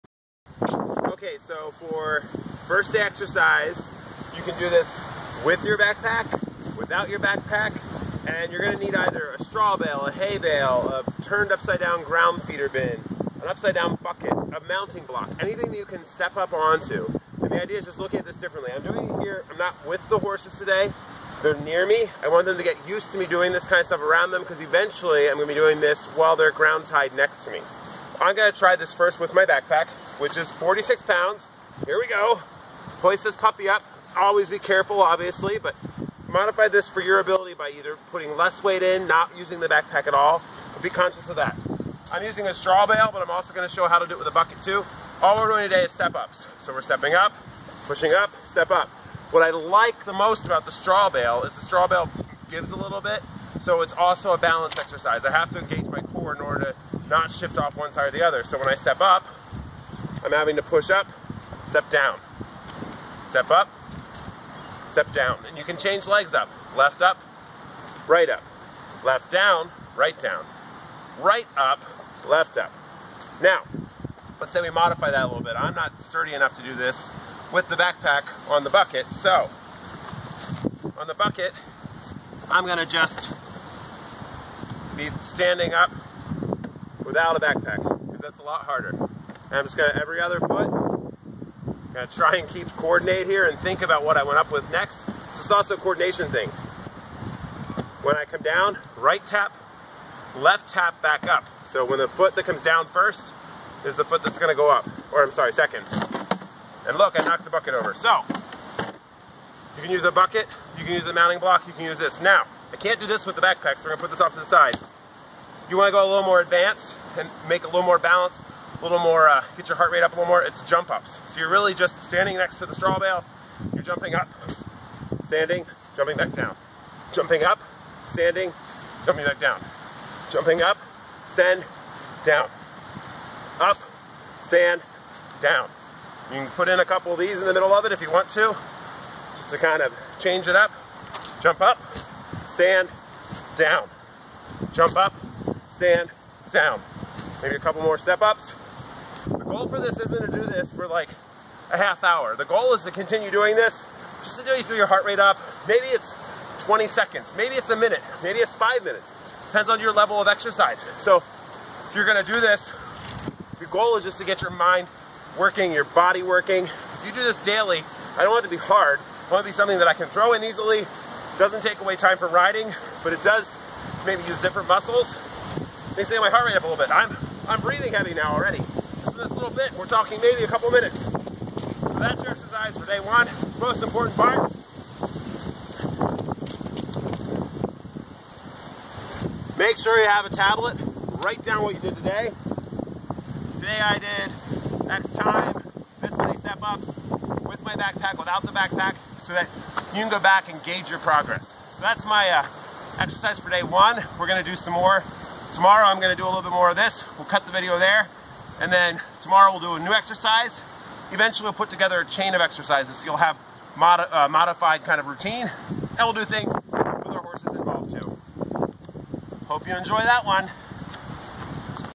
Fit2Ride Challenge Exercise #1
Exercise #1 for the May Fit2Ride Challenge: Step ups and/or jump ups. Eventually we will be doing these with our horses to practice their patience at the mounting block, but for now we'll do the exercise on our own.

You will need:
- Something to step up onto (e.g. straw/hay bale, mounting block, bucket, cinder block)
- Weighted backpack (optional)
- Pen and journal/notebook/calendar

Remember, this isn't about tiring yourself out or working out until exhaustion; it's about raising your heart rate and keeping it realistic so that you don't get burned out but rather stay motivated to do something small everyday.